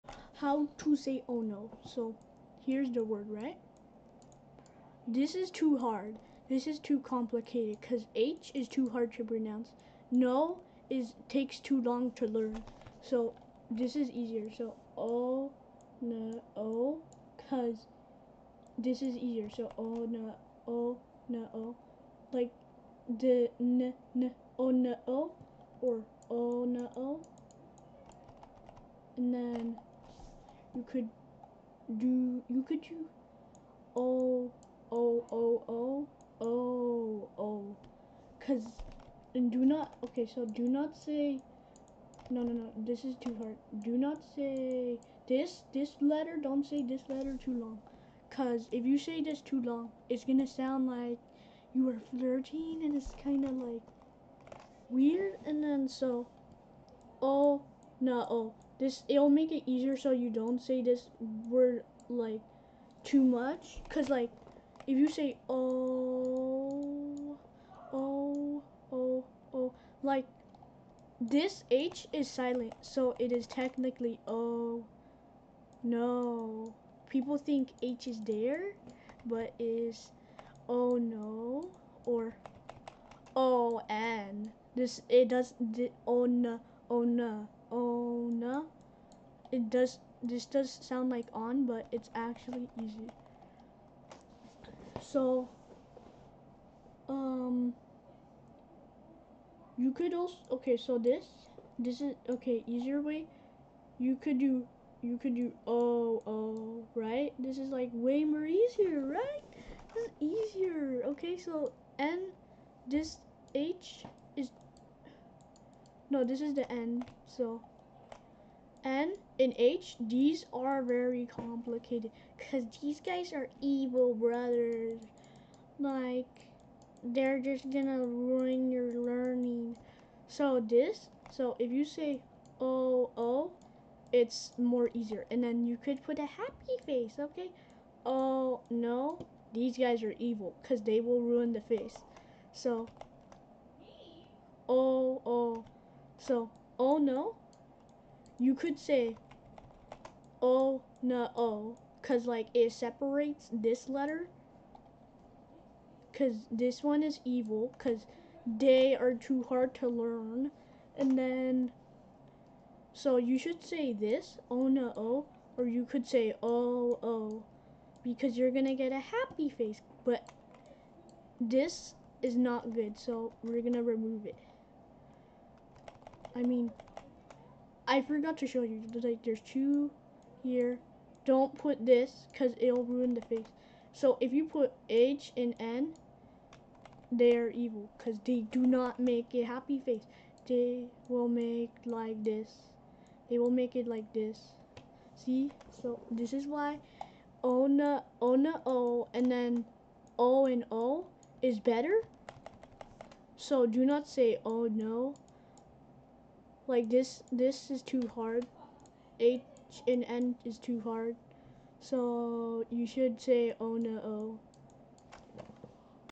how to say oh no/easier way
easy way to say oh no trust me and this is the reason why oh no sounds negative because of h and n